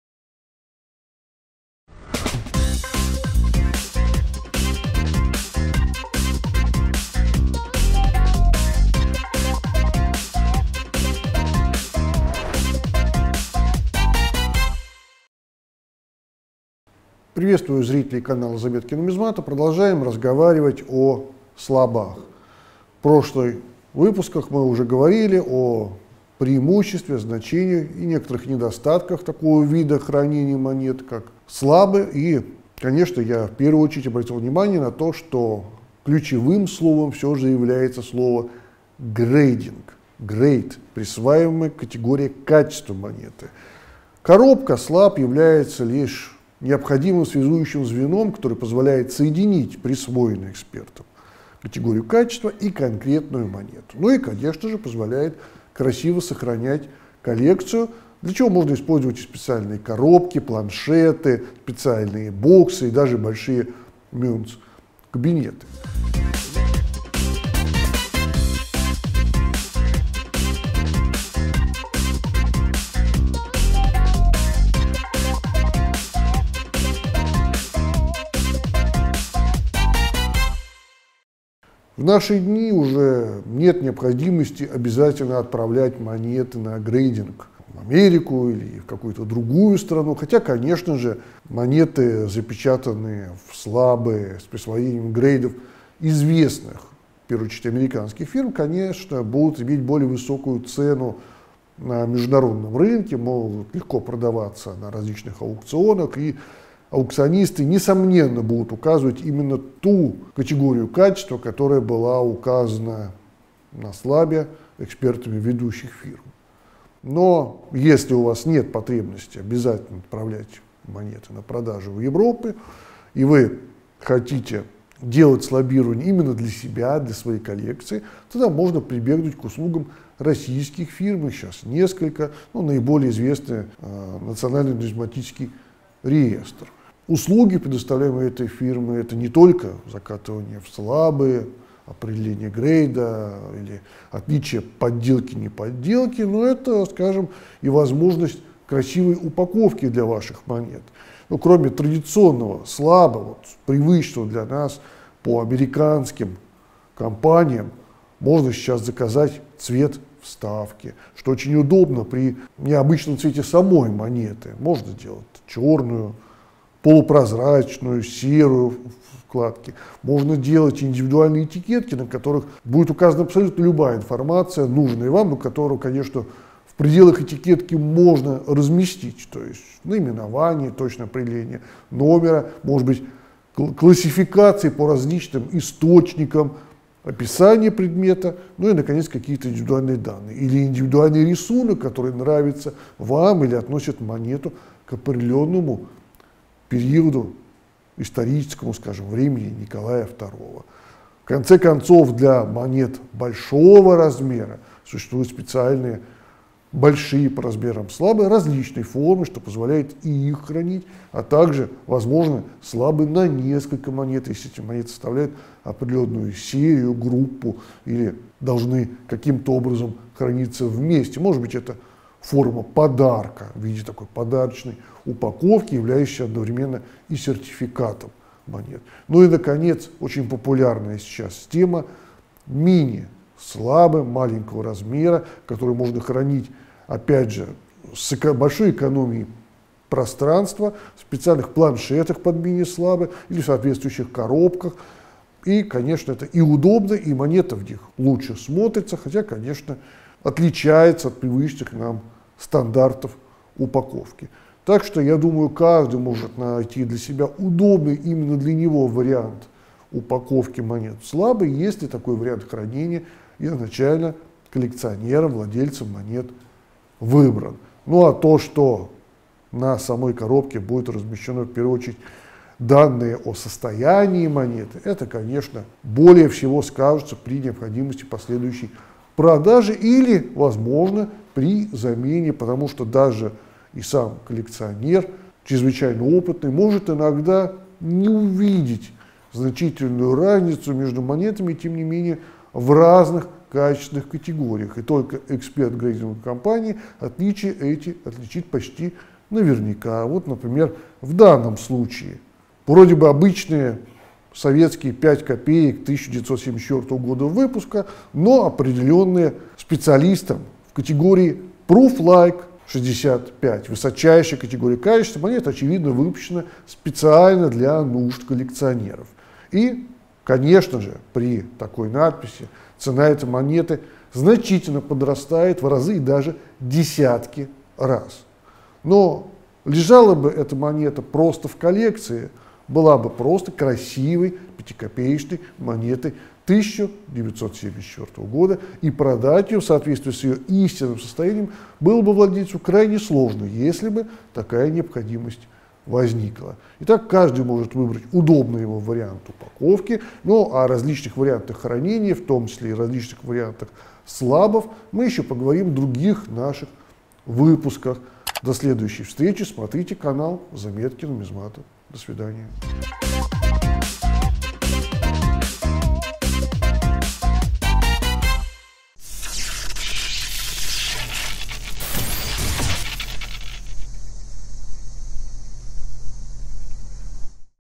Продолжение разговора о хранении монет.
Продолжение разговора о хранении монет в слабах. Разговор ведет Дмитрий Юрьевич.
  - АО "ТрансСервисЭнерго"- надежный поставщик
   электроэнергии.

Профессиональная помощь и консультации:

Наш адрес:
 119136 Москва, 3-й Сетуньский проезд, д.10